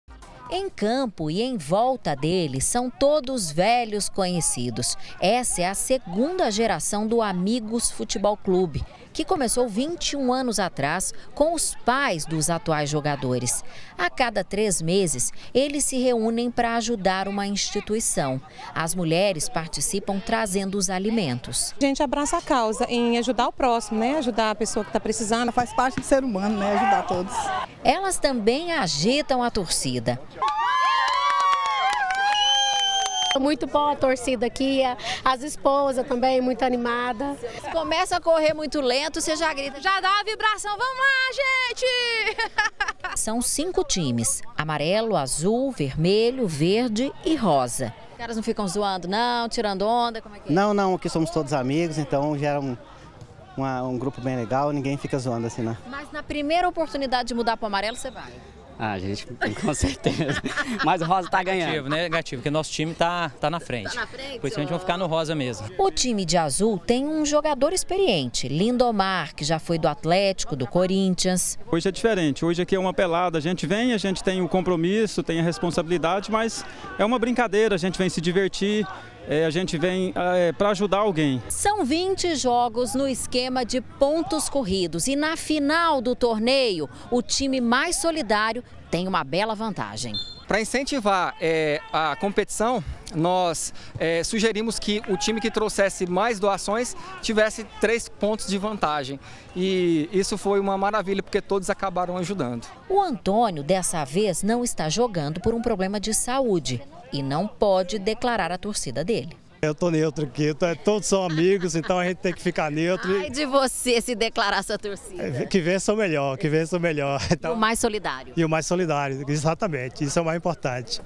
GA - Esporte e solidariedade - 20-03-2018
O grupo "Amigos futebol clube" se reuniu para fazer o primeiro jogo beneficente do ano. Além de mostrar um bom desempenho dentro de campo, os jogadores precisam se mobilizar para conseguir doações de alimentos para entidades carentes. É uma mistura de  esporte e solidariedade que dá muito certo.